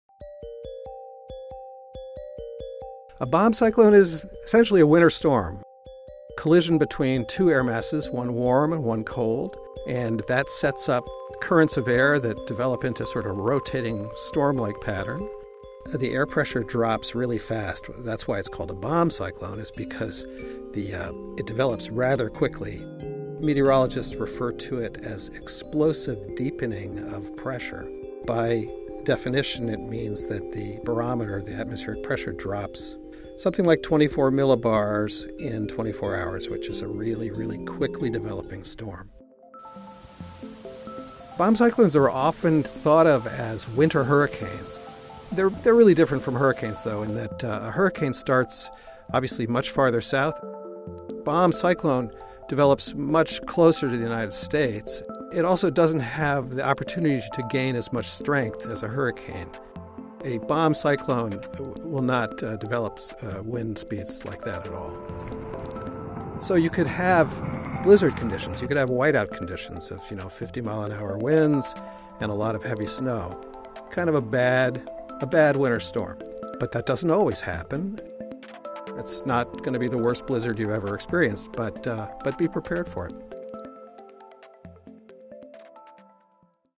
What Is a ‘Bomb Cyclone’? Explaining the Winter Storm
Bomb cyclones have been referred to as “winter hurricanes.” Our science reporter explains how they really work.